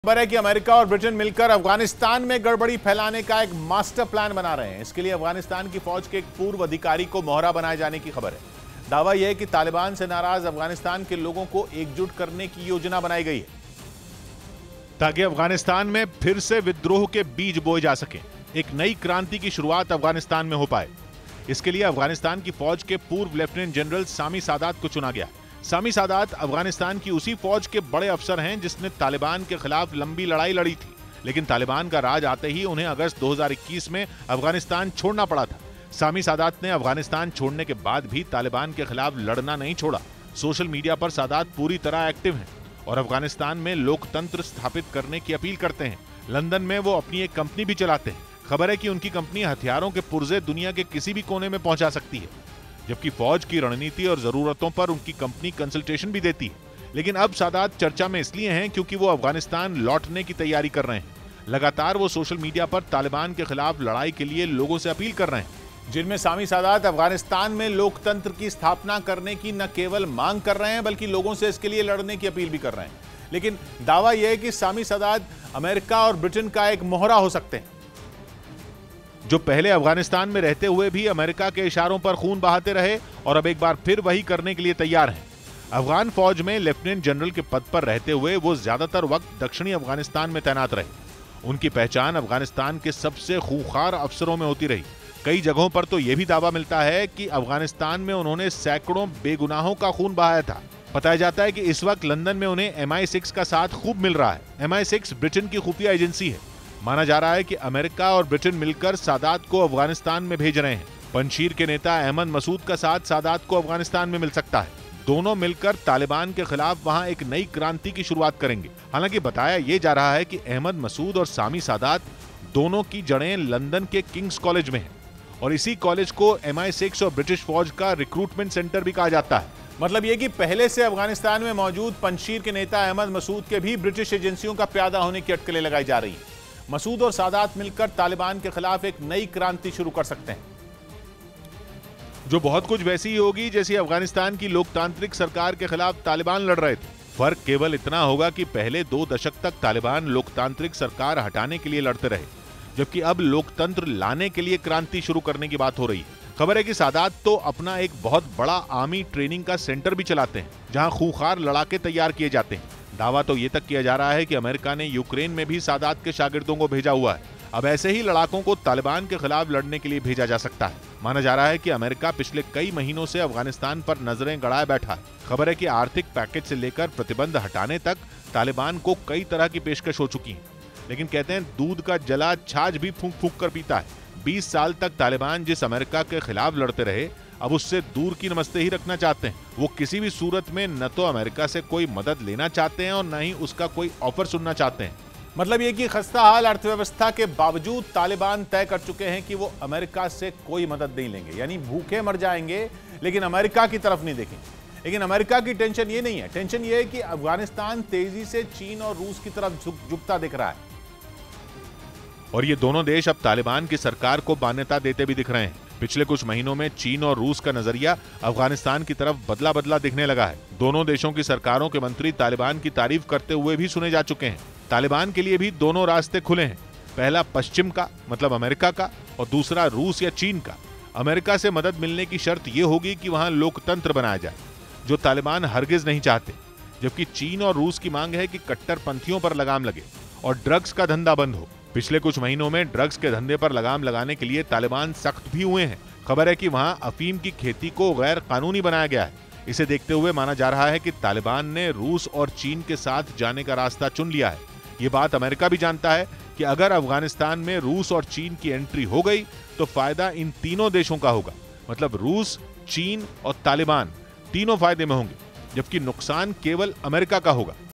America और Britain का अफगानिस्तान में जंग का प्लैन | Talking Point With Kishore Ajwani
अमेरिका और ब्रिटेन मिलकर अफ़ग़ानिस्तान में गड़बड़ी फैलाने का एक मास्टर प्लैन बना रहे हैं। इसके लिए अफ़ग़ानिस्तान की फ़ौज के एक पूर्व अधिकारी को मोहरा बनाए जाने की ख़बर है। दावा ये है कि तालिबान से नाराज़ अफ़ग़ानिस्तान के लोगों को एकजुट किया जाए।